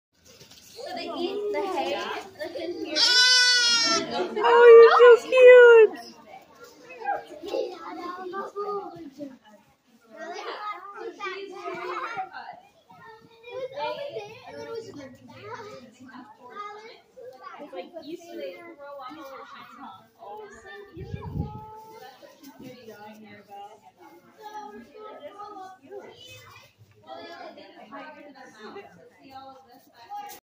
Those little noises🥰
The little lambs at Gale woods farm are just adorable!!